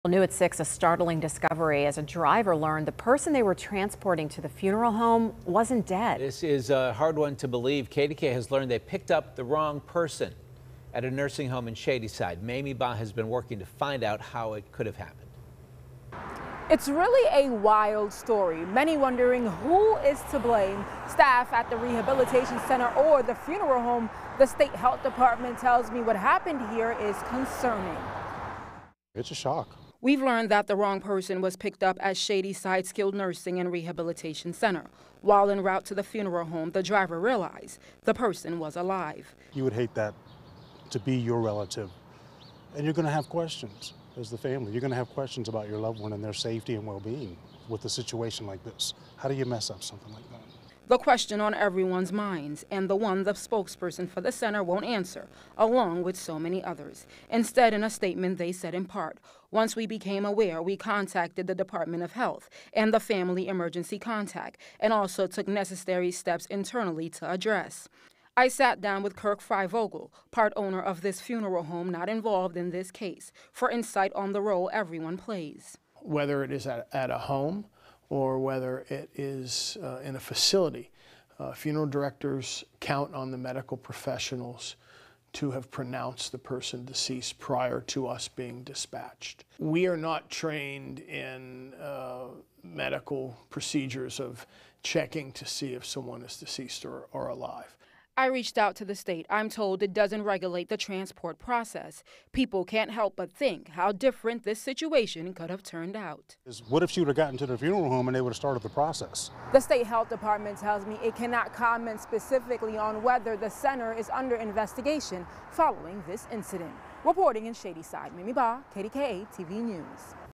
Driver transporting body to funeral home finds the person alive
A driver learned the person they were transporting to the funeral home was not dead.